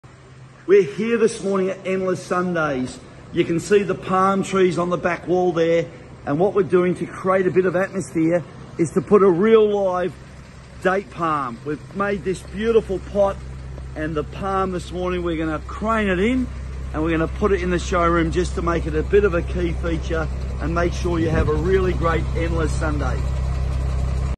Endless Sunday Palm Install
Experience the Palm Tree Aesthetic of Endless Sunday's showroom with our stunning mature Canary Island Date Palm. Complete with a custom metal planter box showcasing their branding, this is the perfect addition to any space.